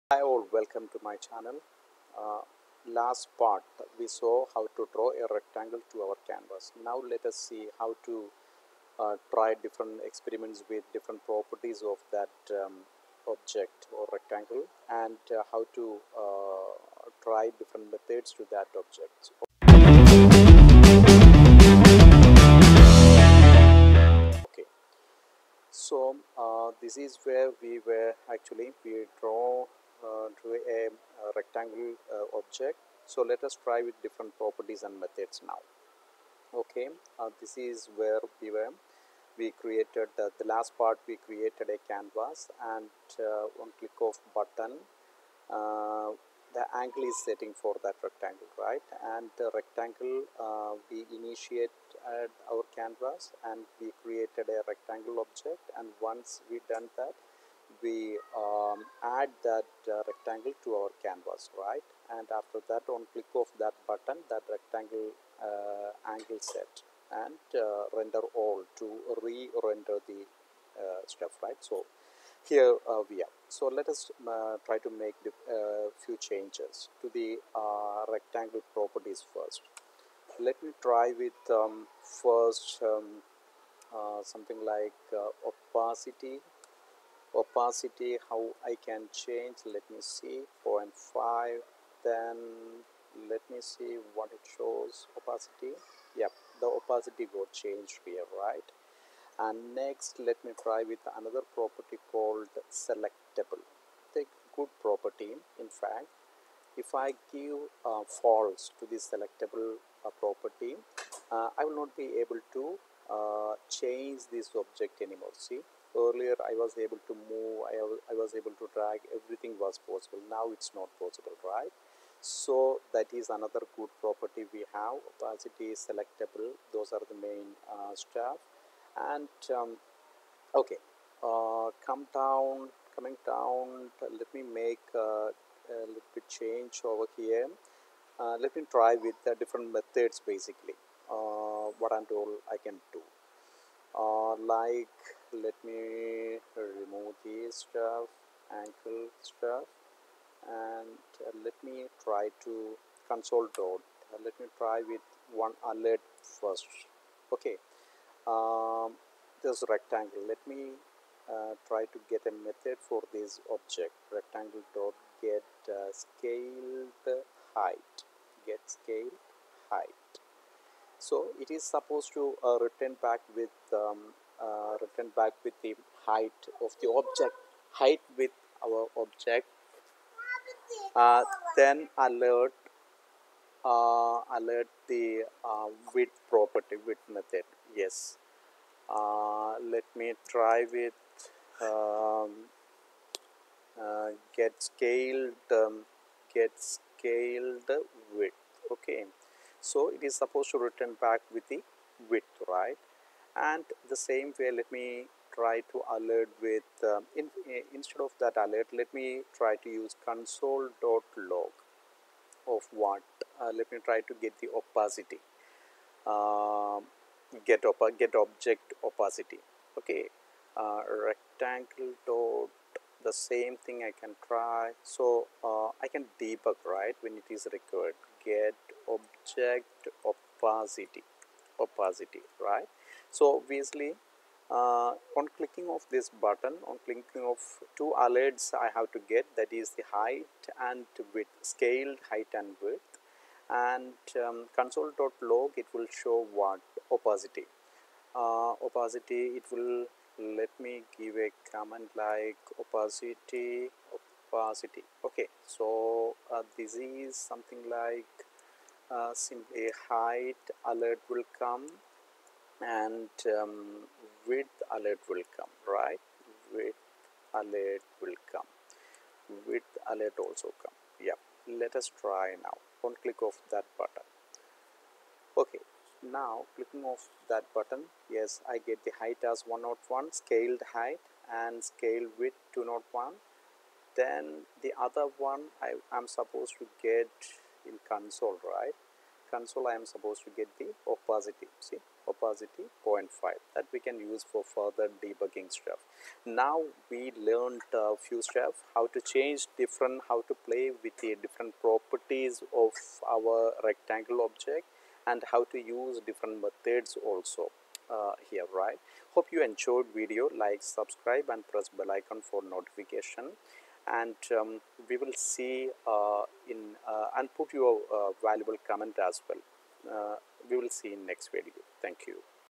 Fabric.js Tutorial: Part 3 - Properties and methods of an object |2021|Easy
This tutorial will help you to understand how to play with different properties and methods of an object